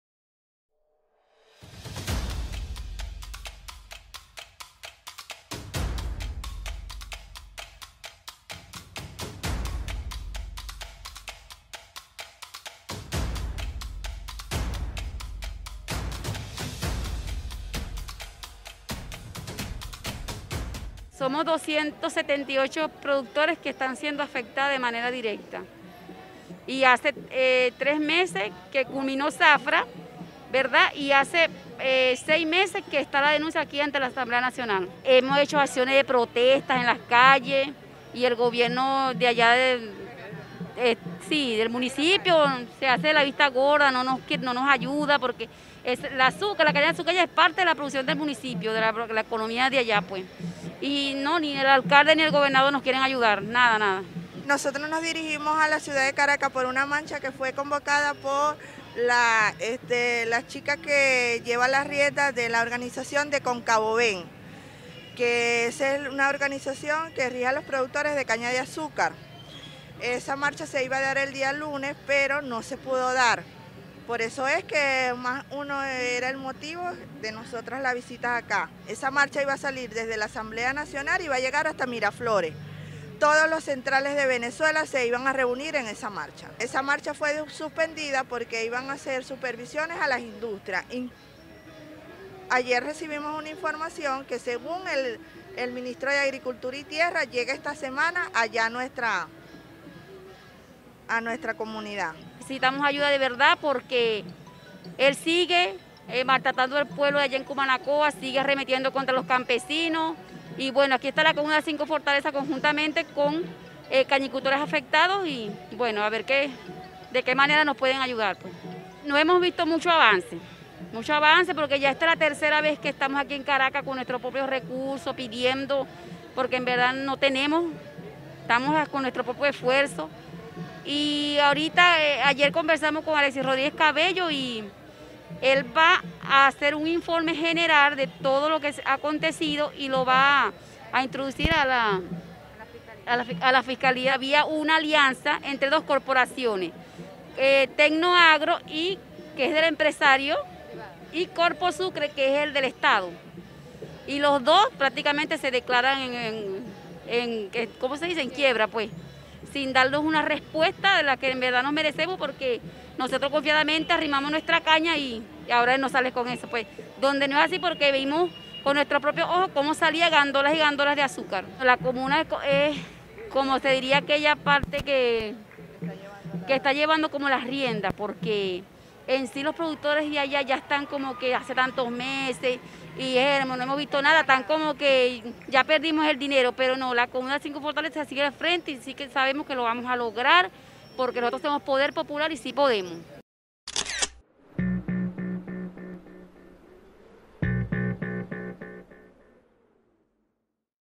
[DENUNCIA] La Corporación Tecno-Agro no responde a cañicultores de Cumanacoa
Una delegación de cañicultoras proveniente de Cumanacoa, Estado Sucre, asistió nuevamente a la Asamblea Nacional para exponer la problemática que atraviesan las y los productores de caña de esta localidad desde abril de 2021.

Y es que a la fecha, no cuentan con respuesta alguna, luego que el Central Azucarero de Cumanacoa, administrado por CorpoSucre, en "Alianza Estratégica" con la Corporación TecnoAgro, se negara a pagar la zafra de 21mil toneladas de caña de azúcar que los productores arrimaron al central, acarreándoles pérdidas por el orden de los 500 mil dólares. El pueblo cañicultor de Cumanacoa sigue movilizado, exigiendo respuesta a la empresa y procurando apoyo por parte de las instituciones venezolanas.